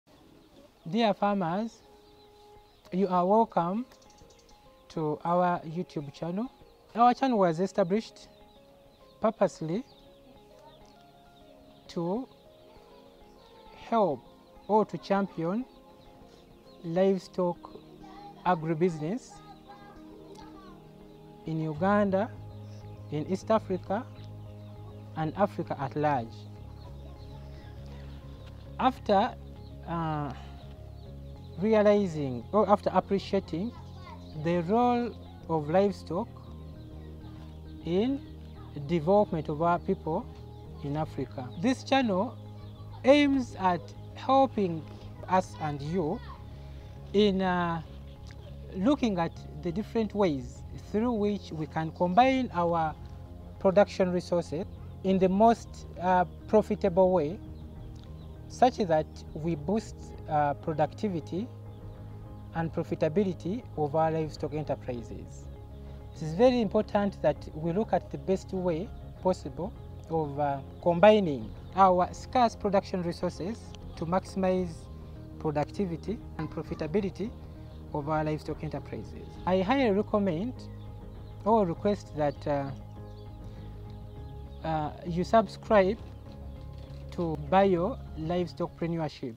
A welcome statement to the Bio Livestockpreneurship You-tube channel
The Bio Livestockprenuership you tube channel is one of the communication platforms established by Bio-LMS. The Bio-LMS is a young and dynamic company providing innovative solutions (services and products) that add value to farmers’  livestock  ventures/enterprises all aimed at sustainable  production, productivity and commercialization of the livestock industry through;nutrition, health, genetics, management performance and partnerships.